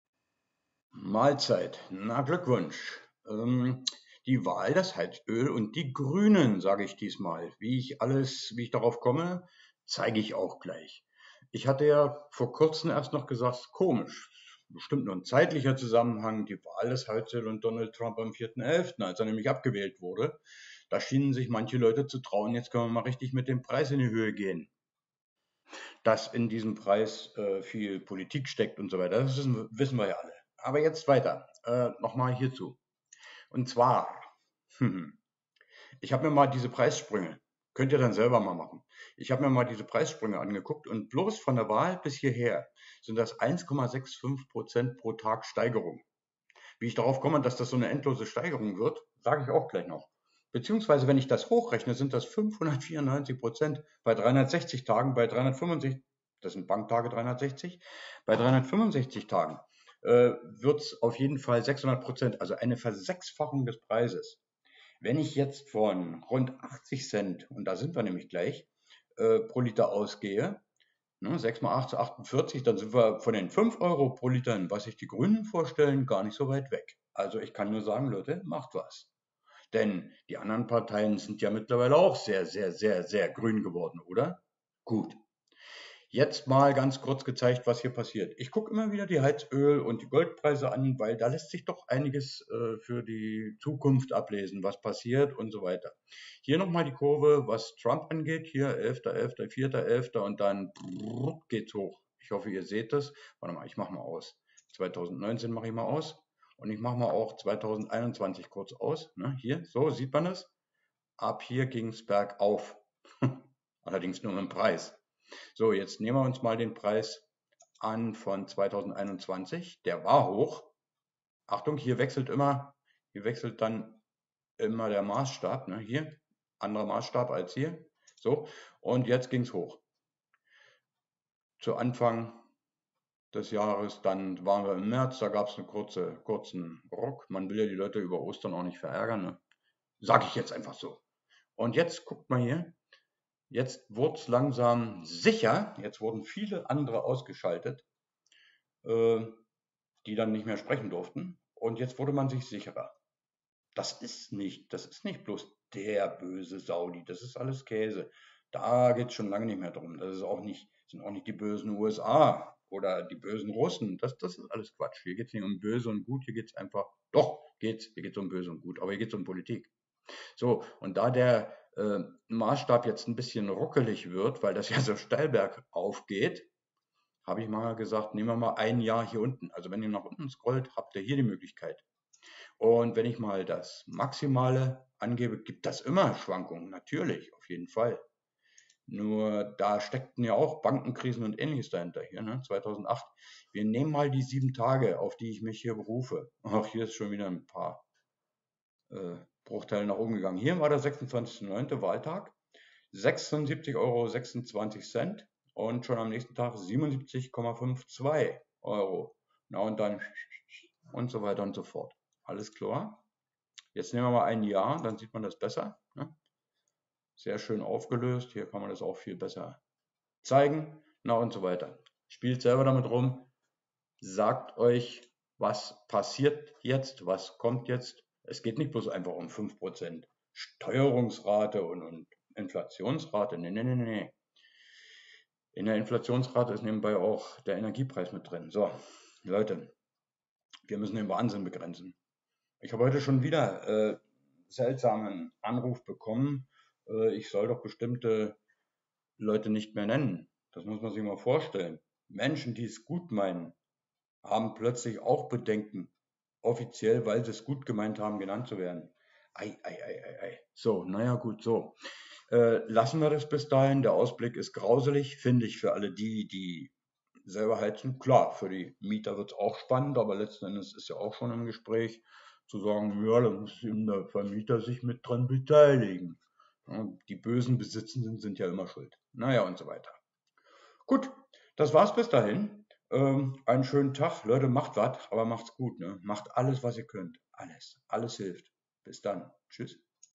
Die Wahl, das Heizöl und die Grünen mit 5€ pro Liter
+++ Meinungsfreiheit im MaPhy-Forum +++ Einfachste Mathematik sagt es voraus: Inflation. 5% ? Nein! Bildung hilft.

Björn Köhler